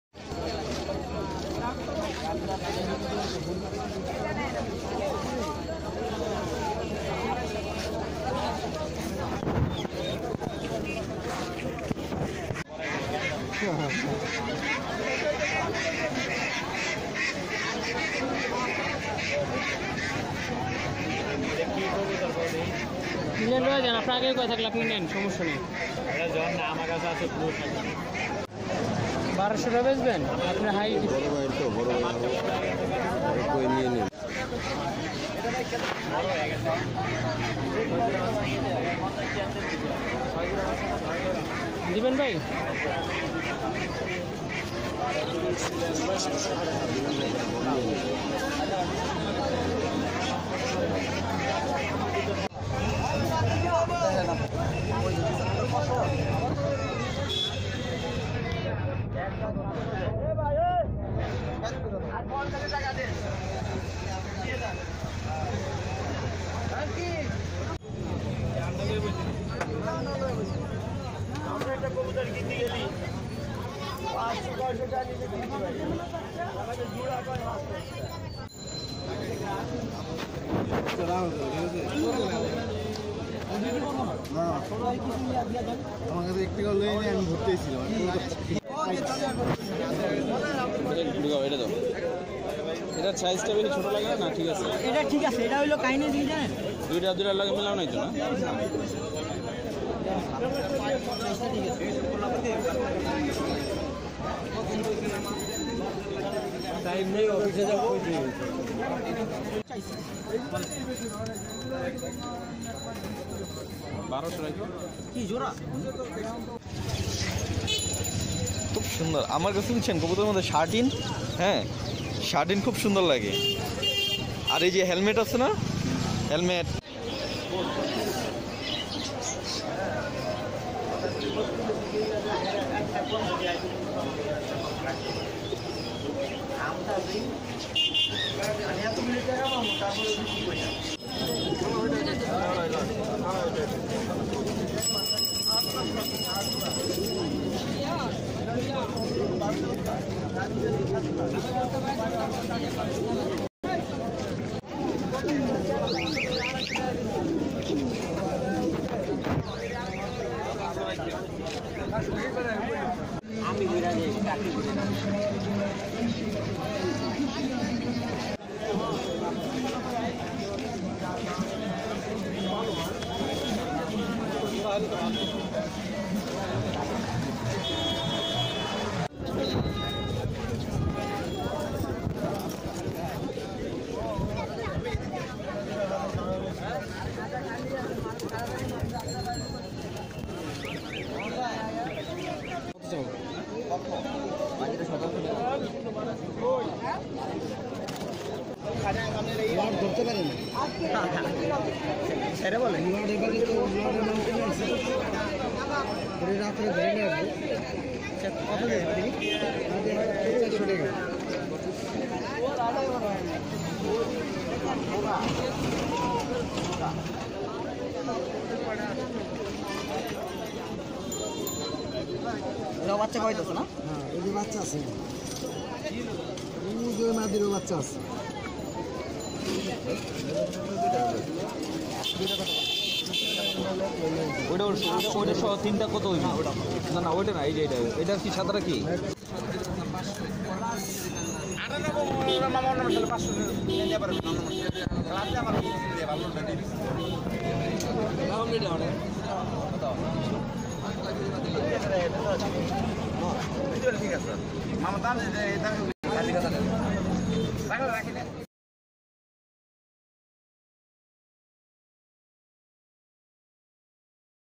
Pigeon market in Dhaka | কবুতরের দাম | pigeons price video
In Dhaka city Pigeon Market of Bangladesh is an amazing place for buy pigeons.Which is also known as kobutor or pigeon hat.